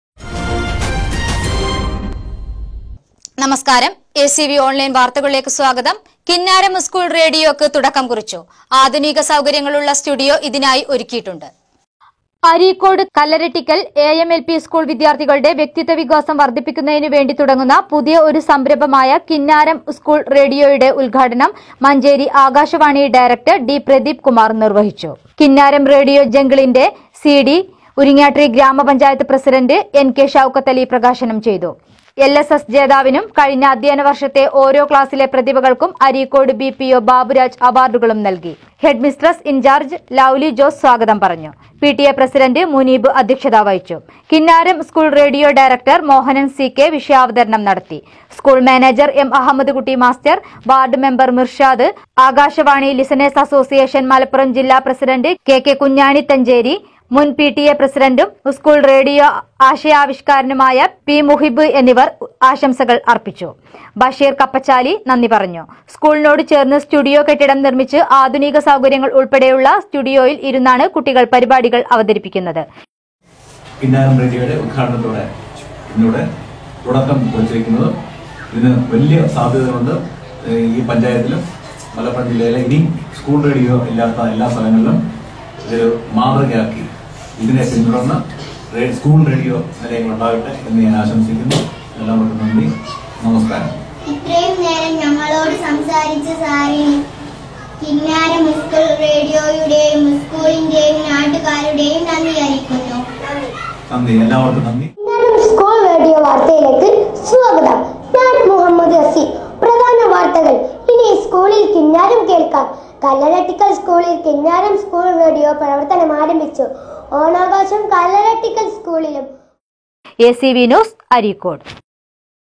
കിന്നാരം റേഡിയോ പ്രക്ഷേപണം ആരംഭിച്ചു
ഊർങ്ങാട്ടിരി കല്ലരട്ടിക്കൽ എ എം എൽ പി സ്കൂളിൽ കിന്നാരം റേഡിയോ പ്രക്ഷേപണം തുടങ്ങി